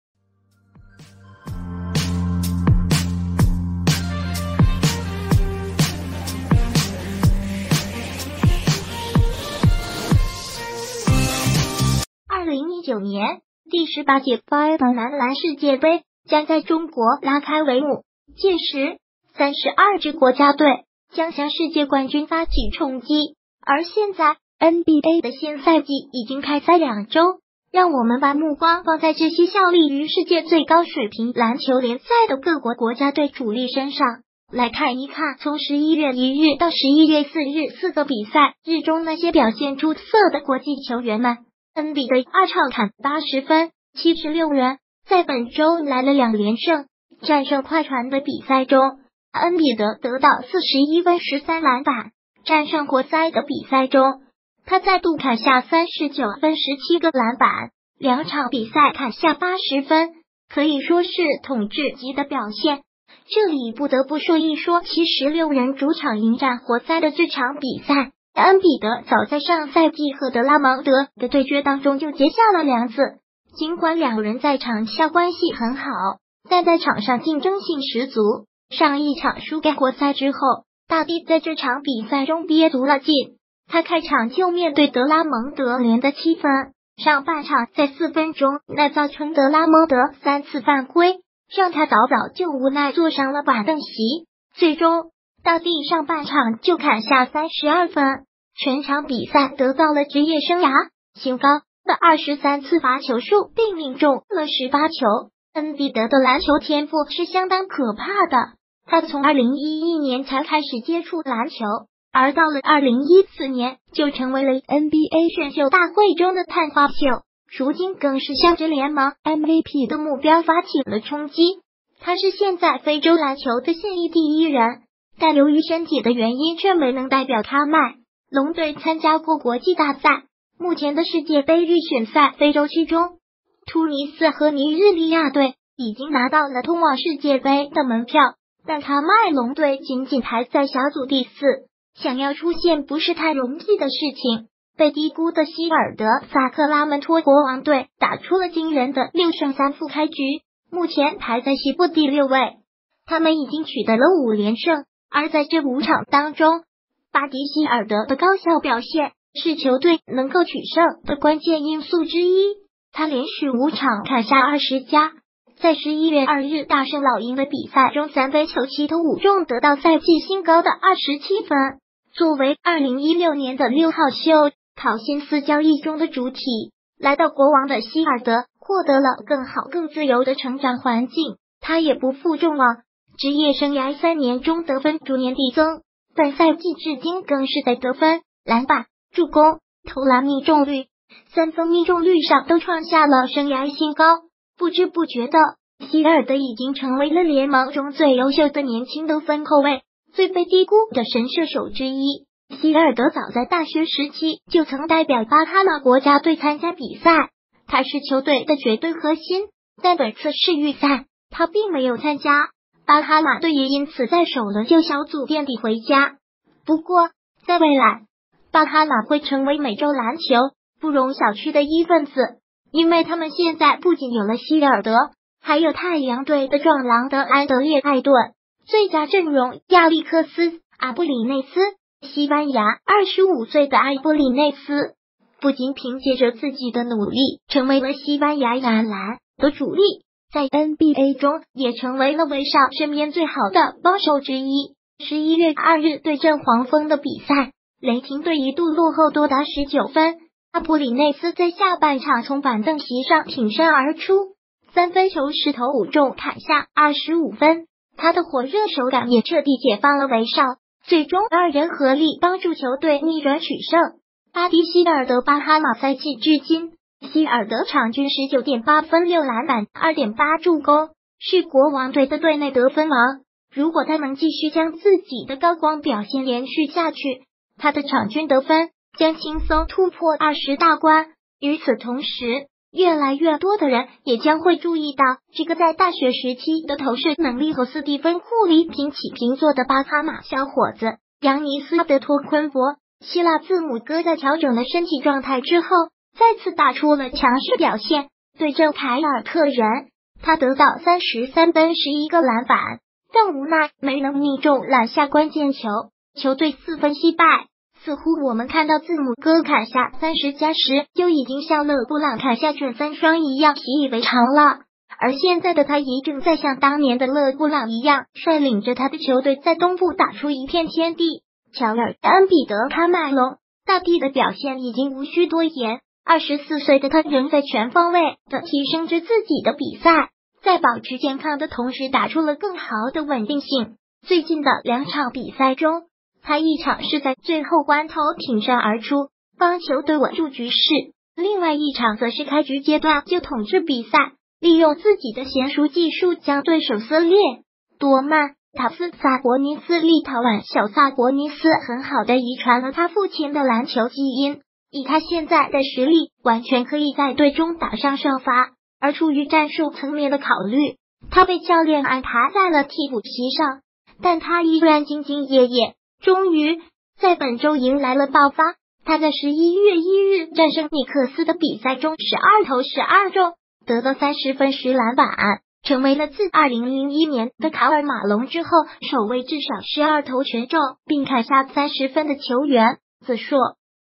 NBA国际热榜：大帝2场80分 巴哈马之子连续5场20+
2019年，第18届FIBA男篮世界杯将在中国拉开帷幕。届时，32支国家队将向世界冠军发起冲击。而现在，NBA的新赛季已经开赛两周，让我们把目光放在这些效力于世界最高水平篮球联赛的各国国家队主力身上，来看一看从11月1日到11月4日4个比赛日中那些表现出色的国际球员们。恩比德2场砍80分76人在本周迎来了2连胜，战胜快船的比赛中，恩比德得到41分13篮板；战胜活塞的比赛中，他再度砍下39分17个篮板，两场比赛砍下80分，可以说是统治级的表现。这里不得不说一说76人主场迎战活塞的这场比赛。恩比德早在上赛季和德拉蒙德的对决当中就结下了梁子，尽管两人在场下关系很好，但在场上竞争性十足。上一场输给活塞之后，大帝在这场比赛中憋足了劲，他开场就面对德拉蒙德连得7分，上半场在4分钟内造成德拉蒙德3次犯规，让他早早就无奈坐上了板凳席。最终，大帝上半场就砍下32分，全场比赛得到了职业生涯新高的23次罚球数并命中了18球。恩比德的篮球天赋是相当可怕的，他从2011年才开始接触篮球，而到了2014年就成为了NBA选秀大会中的探花秀，如今更是向着联盟MVP的目标发起了冲击。他是现在非洲篮球的现役第一人，但由于